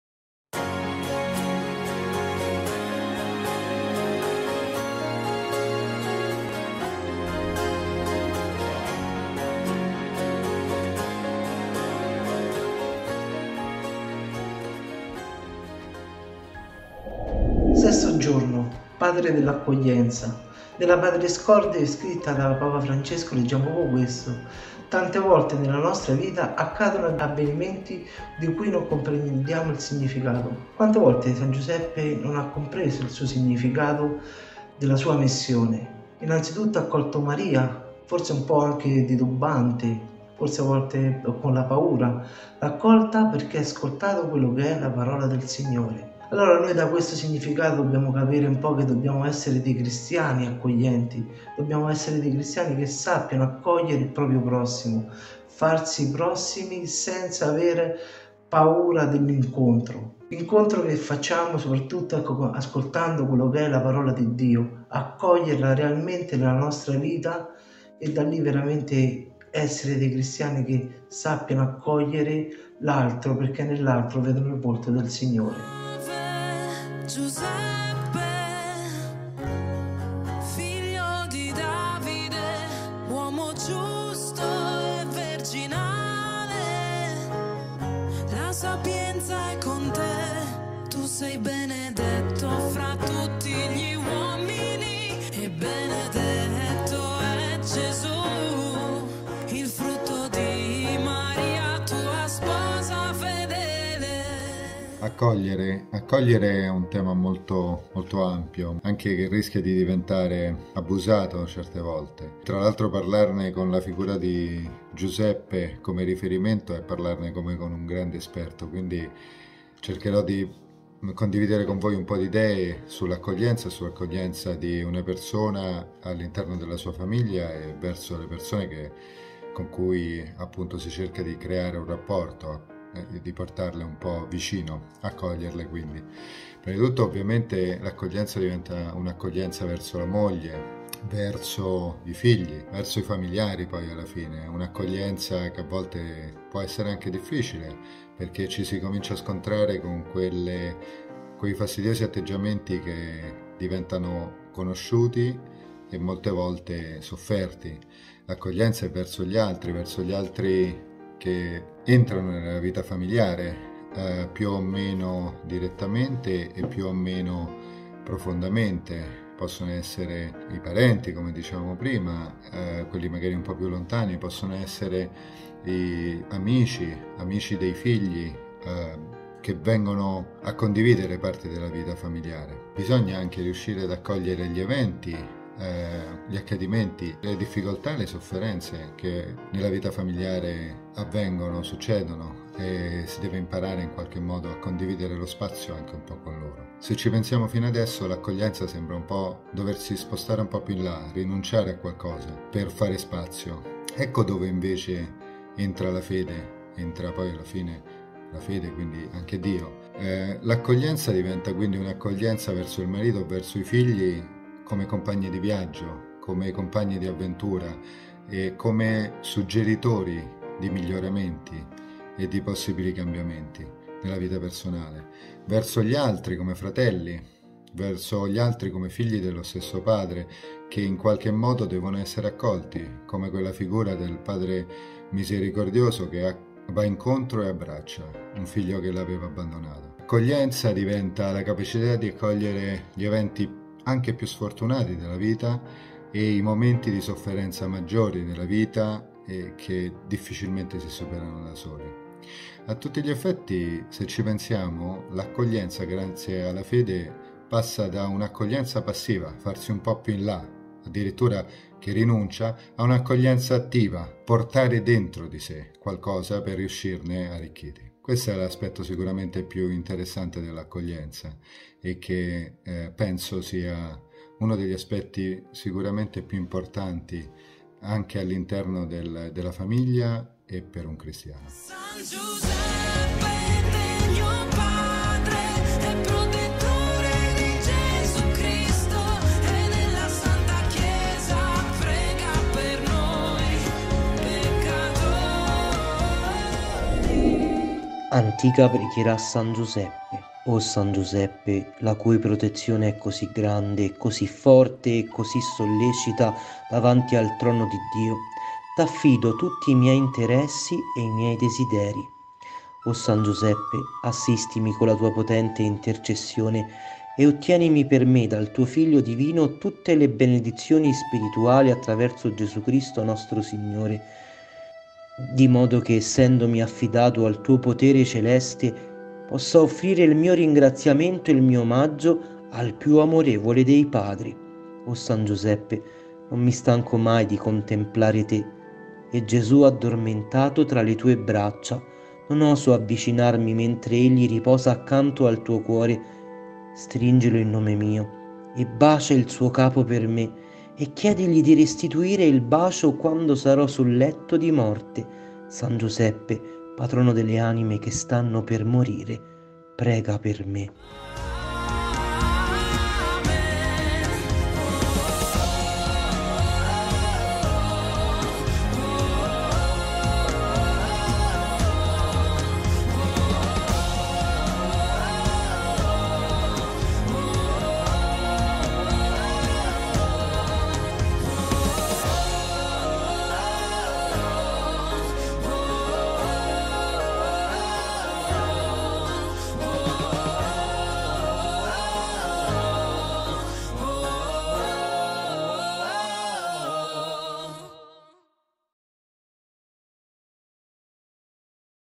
Novena a San Giuseppe - Giorno 6 15.03.2021
Padre nell’accoglienza.

“Tante volte, nella nostra vita, accadono avvenimenti di cui non comprendiamo il significato. La nostra prima reazione è di delusione e ribellione. Giuseppe lascia da parte i suoi ragionamenti e fa spazio a ciò che accade e lo accoglie.” Dalla Lettera Apostolica PATRIS CORDE del Santo Padre Francesco